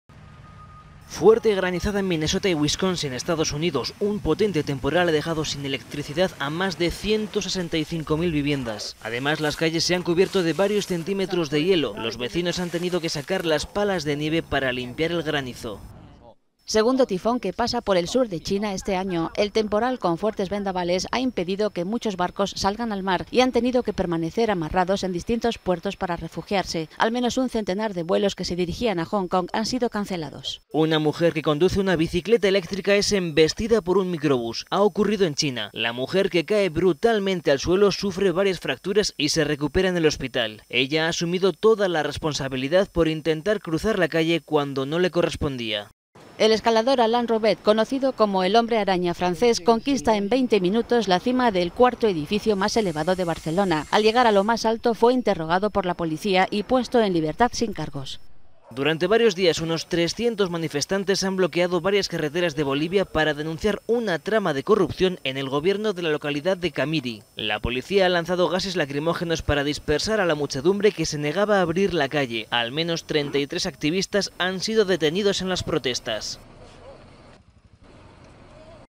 Otras noticias del mundo
Alain Robert, el llamado Spiderman francés, ha escalado el cuarto edificio más alto de Barcelona. Y como viene siendo habitual en sus hazañas, ha sido detenido y luego puesto en libertad. Es una de las noticias de nuestra crónica de sucesos. También la vuelta del invierno a Minnesota, en EEUU, con una tremenda granizada.